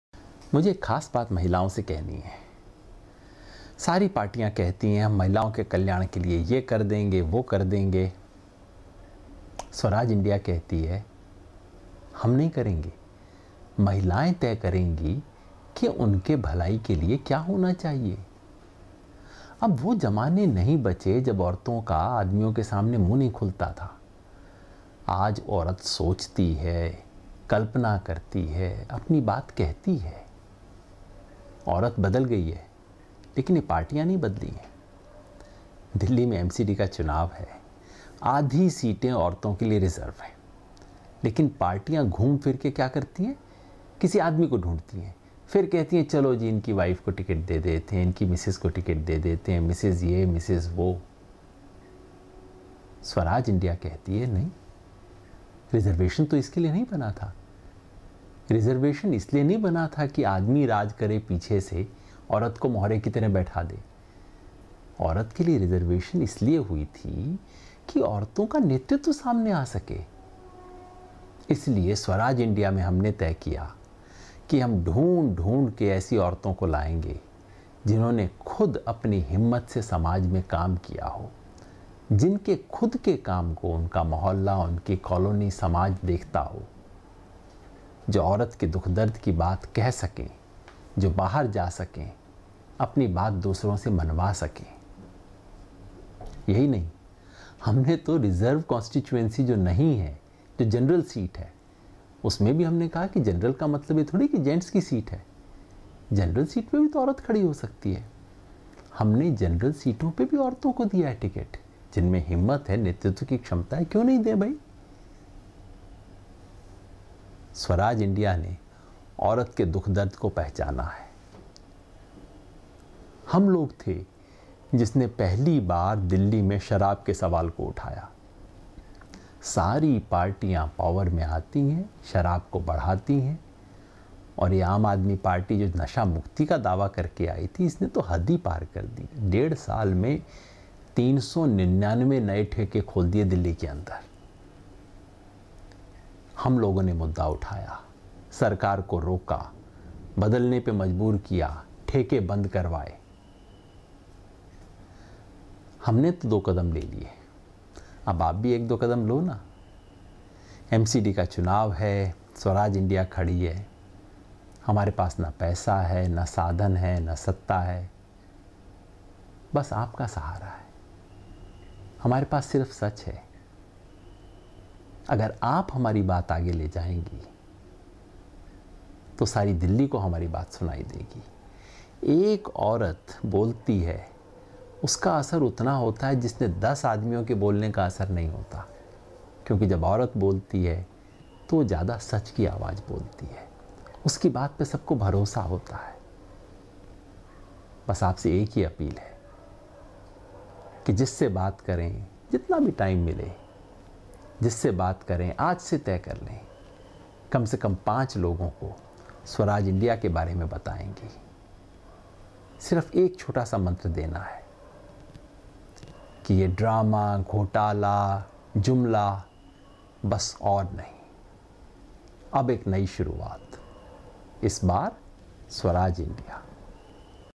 Women in Fray for Delhi MCD - Swaraj India Yogendra Yadav.
Swaraj India MCD Polls Delhi